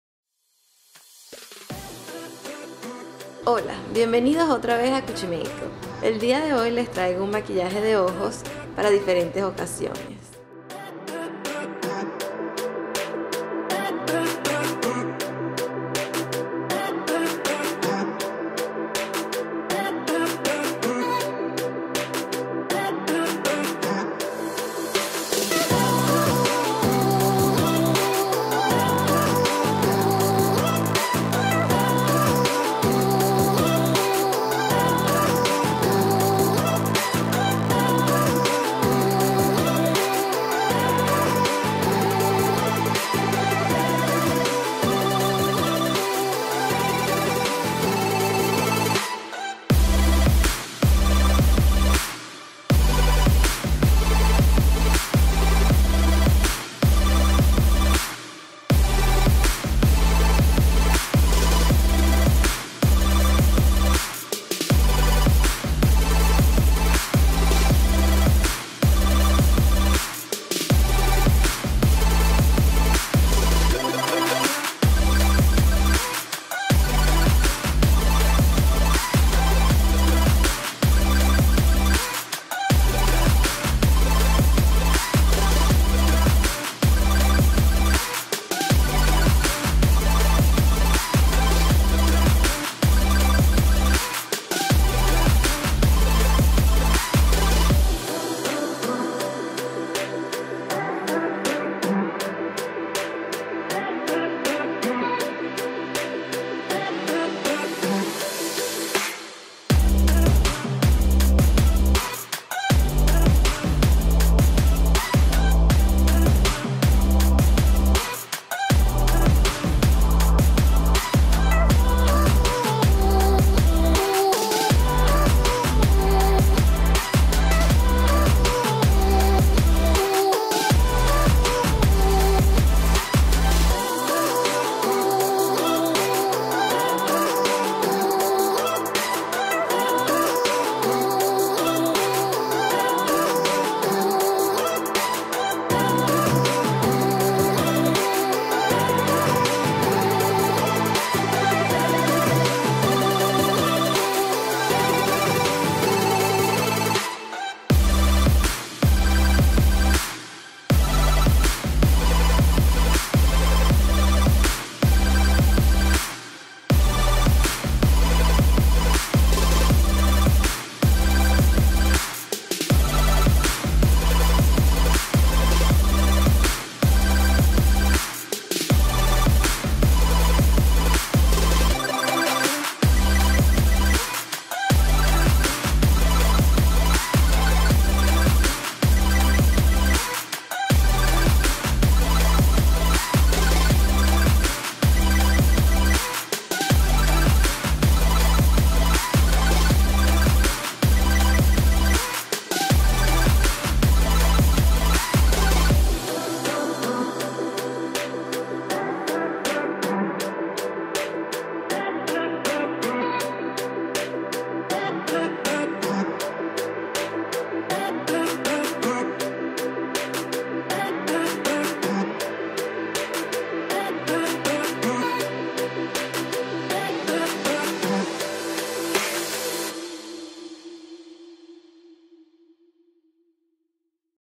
Easy Daytime Makeup | CUCHI MAKEUP
Welcome back! Today's tutorial is perfect for a daytime event, with graduations and weddings coming up this makeup is very easy to recreate and will make a beautiful one during the day. Hope you enjoy!

Products Used:
MAC Pro Longwear Paintpot (Color: Soft Ochre)
Urban Decay Naked Eyeshadow Palette
Urban Decay Naked Smoky Eyeshadow Palette
MAC Powerpoint Eye Pencil (Color: Stubborn Brown)
MAC In Extreme Dimension Lash Mascara (Color: Black Extreme)

Recorded with Canon Rebel T5
Edited By: Gabriela Serrano